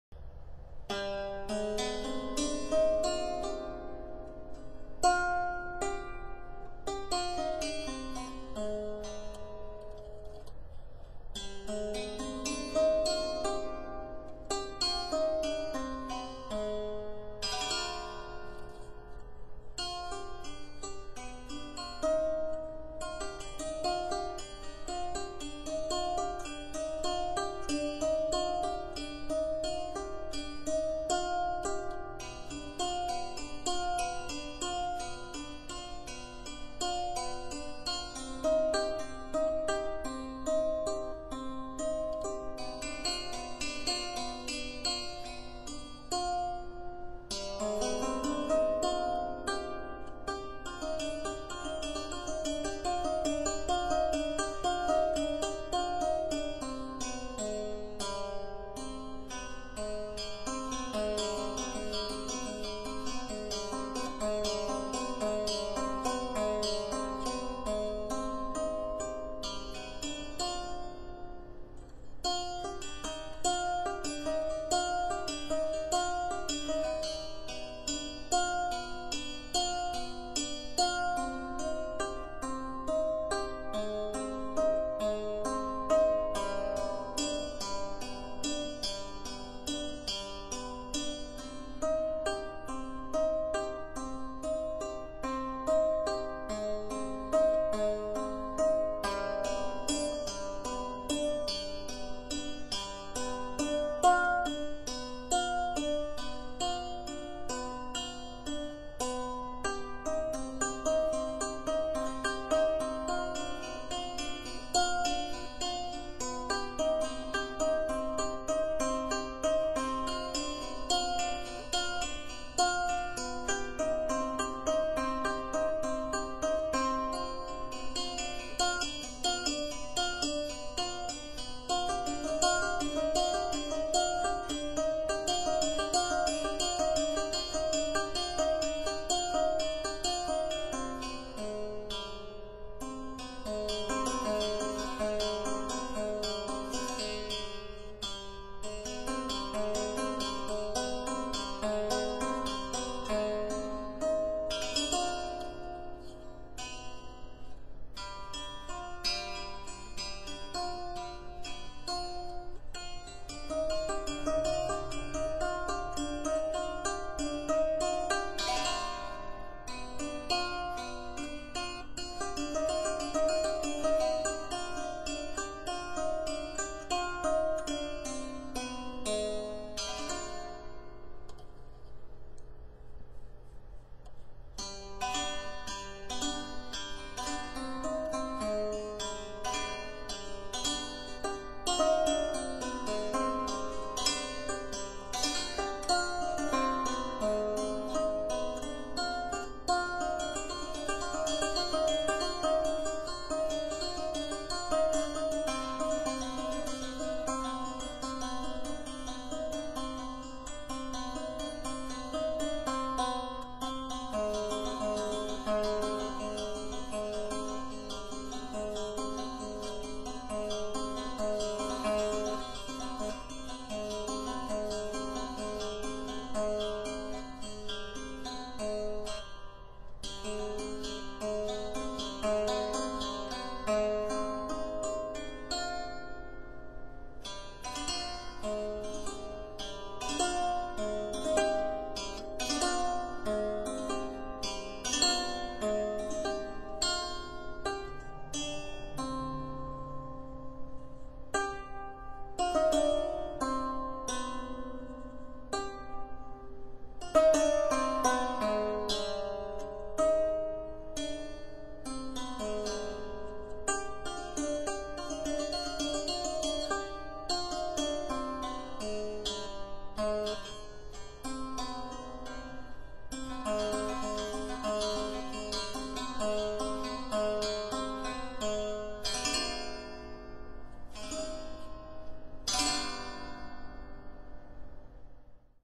Home made 9-stringed tin can cottonwood branch kora in hijaz tuning
Small hand crafted 9-stringed tin can kora. The tin can kora neck is made from an old random cottonwood branch. Steel strings, and zither tuning pins.